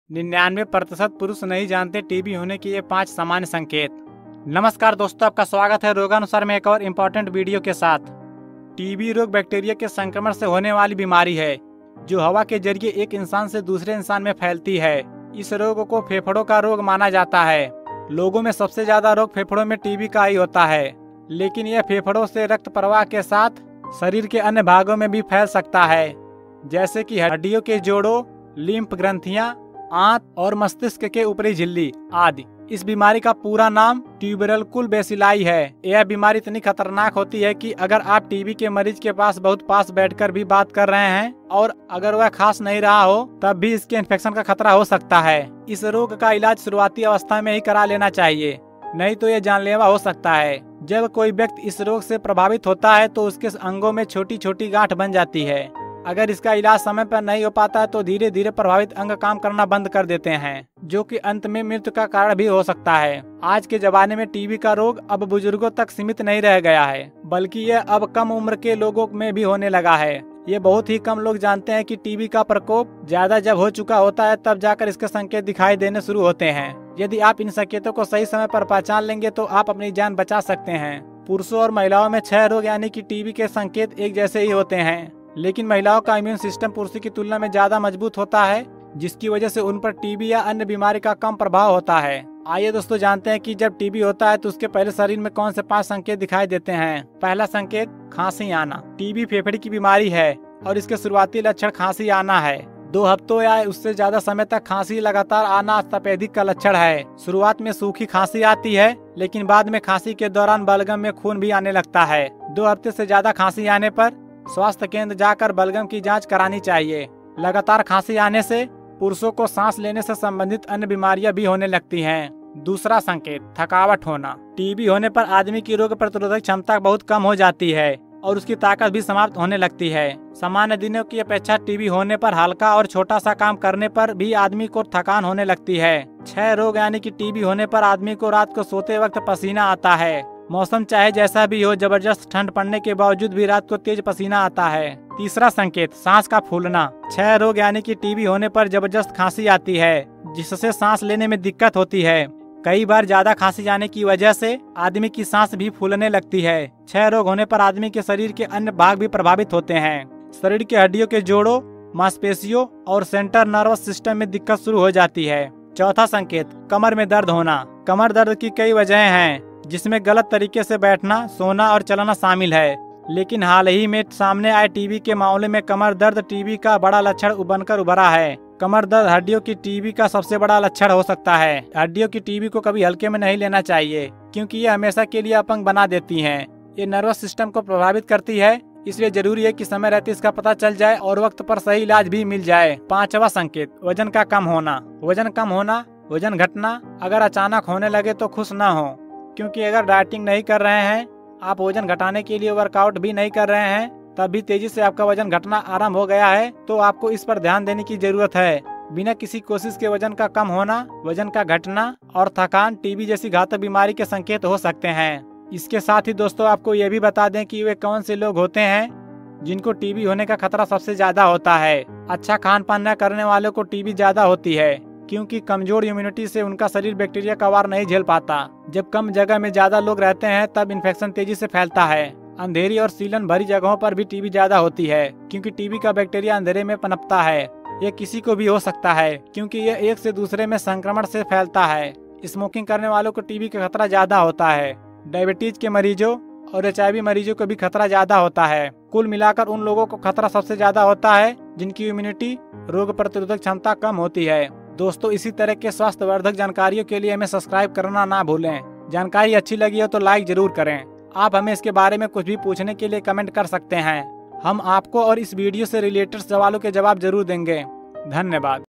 TB होने से पहले शरीर देता है ये 5 संकेत पता नहीं चला तो मौत पक्की, देख लो कहीं बाद मे पछताना ना पड़े
टीबी रोग बैक्टीरिया के संक्रमण से होने वाली बीमारी है, जो हवा के जरिए एक इंसान से दूसरे इंसान में फैलती है। इस रोग को फेफड़ों का रोग माना जाता है। लोगो में सबसे ज्यादा रोग फेफड़ों में टीबी का ही होता है, लेकिन यह फेफड़ों से रक्त प्रवाह के साथ शरीर के अन्य भागों में भी फैल सकता है, जैसे की –  हड्डियों के जोड़, लिम्फ ग्रंथियां, आंत,  त्वचा और मस्तिष्क के ऊपर की झिल्ली आदि।  इस बीमारी का पूरा नाम ट्यूबरकुल बेसिलाइ है। यह बीमारी इतनी खतरनाक है, की अगर आप टीबी के मरीज के बहुत पास बैठकर भी बात कर रहे है और वह खाँस नहीं रहा हो तब भी इसके इंफ़ेक्सन  का खतरा हो सकता है। इस रोग का इलाज़ शुरूआती अवस्था में ही करा लेना चाहिए नहीं तो, यह जानलेवा हो सकता है। जब कोई व्यक्ति इस रोग से प्रभावित होता है, तो उसके अंगों में छोटी-छोटी गांठ बन जाती हैं। अगर इसका इलाज़ समय पर नहीं हो पाता है, तो धीरे-धीरे प्रभावित अंग काम करना बंद कर देते हैं, जो की मृत्यु का भी कारण हो सकता है।